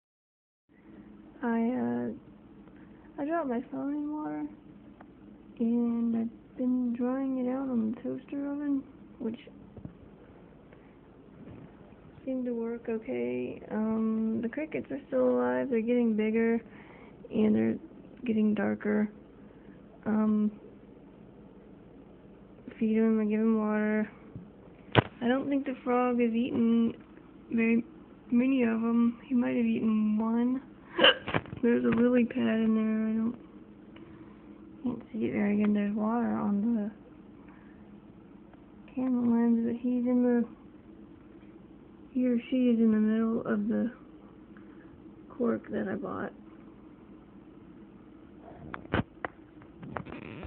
Dropped galaxy phone in water while doin wtrchange
Water on the lense. Took nap didn't feel like getting up.